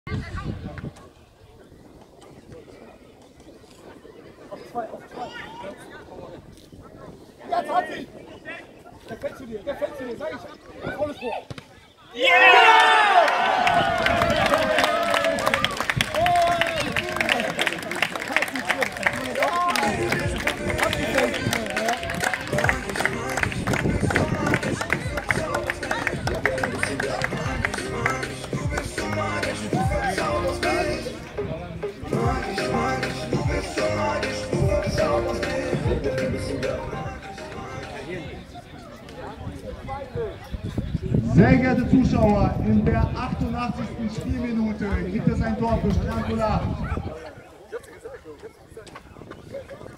NTSV Strand 08 - FC Dornbreite 6:0
Marcel Hansen trifft zum Endstand.